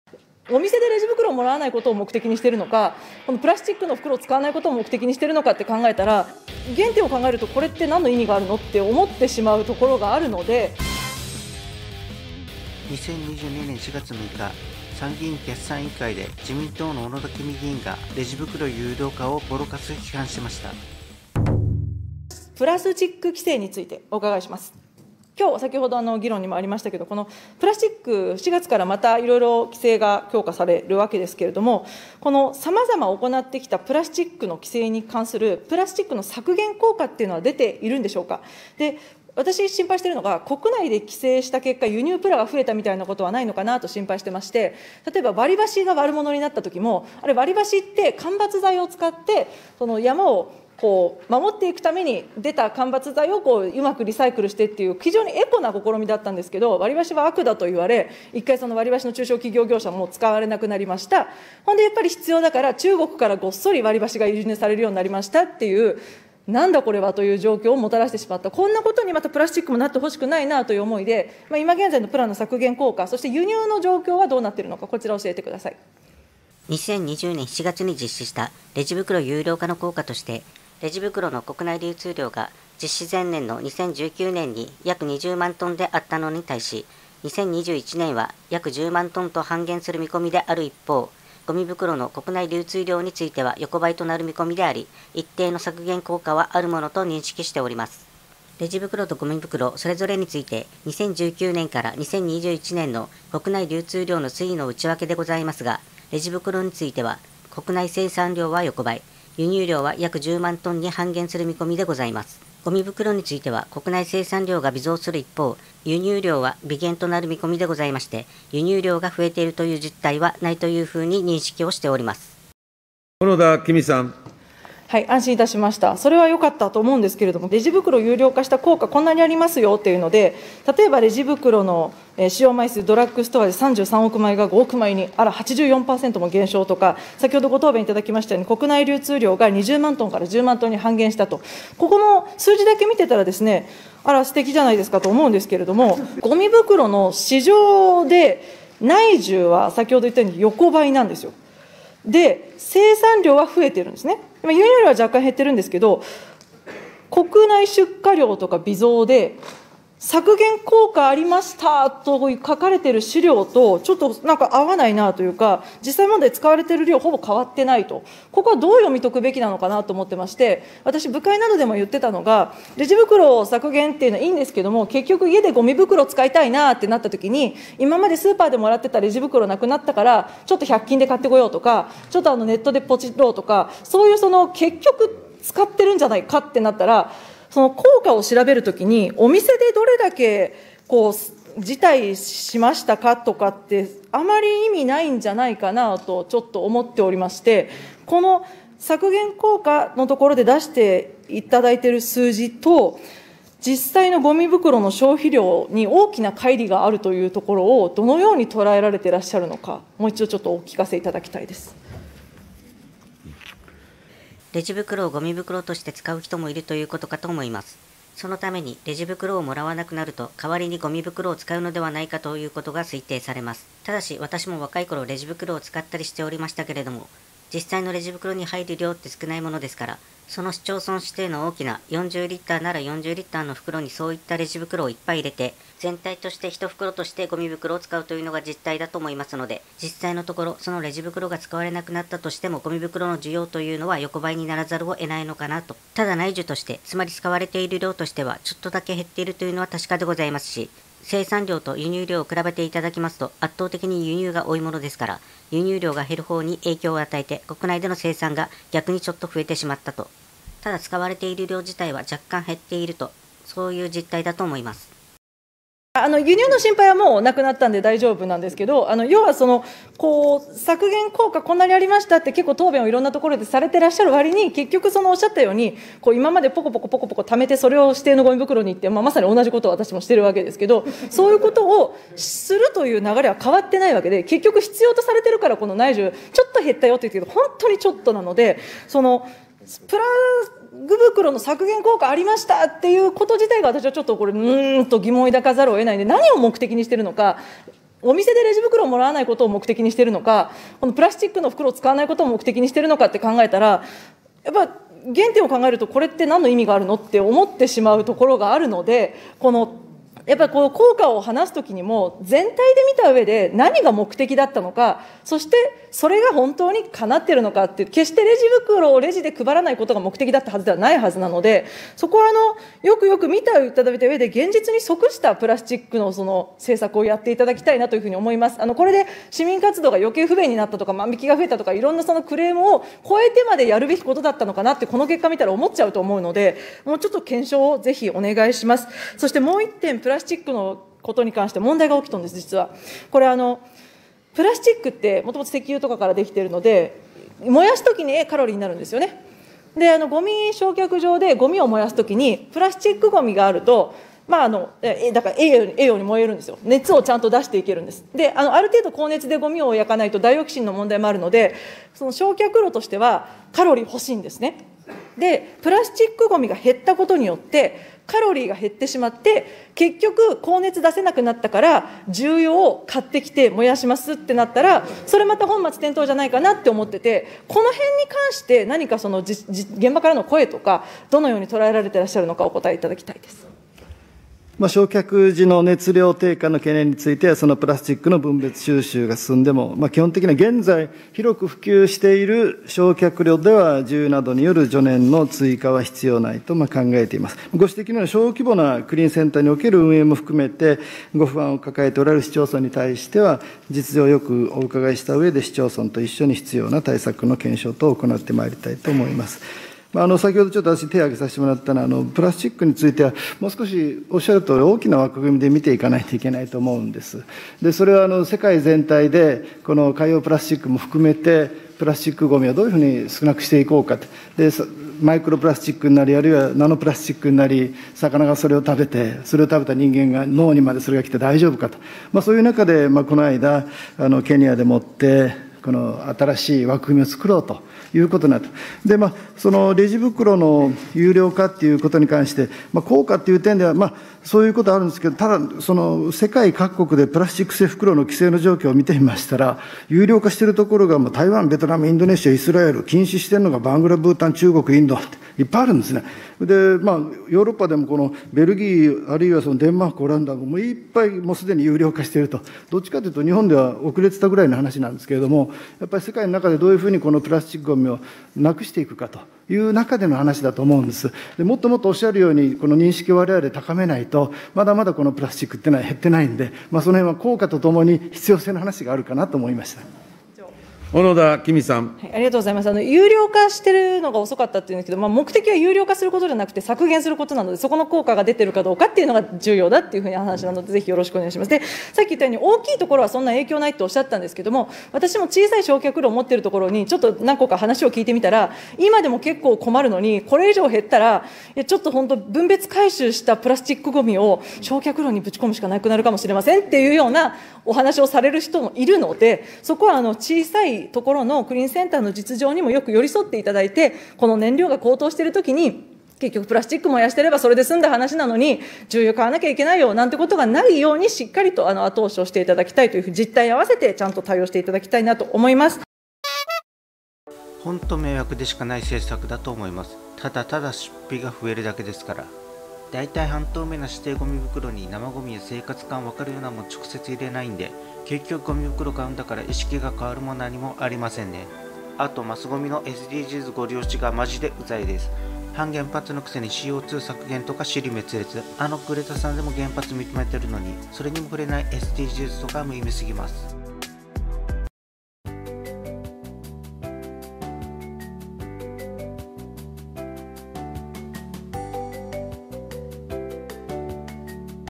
小野田紀美 レジ袋有料化、プラ規制に激おこ批判「何か意味あるの？」
2022年4月6日
参議院決算委員会
自民党　小野田紀美

コメントはコミュニティガイドラインを遵守するようにお願いします